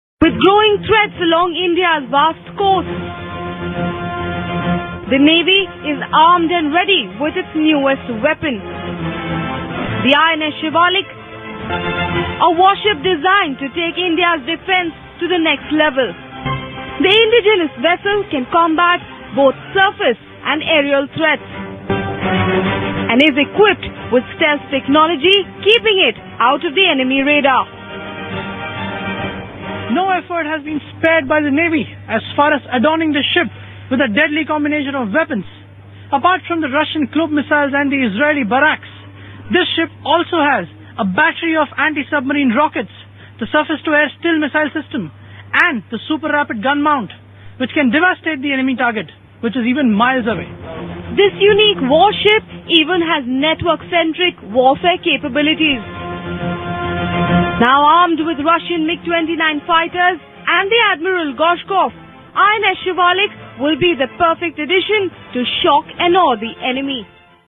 INS Shivalik - India's indigenous stealth warship
INS Shivalik India's 1st indigenous stealth warship commissioned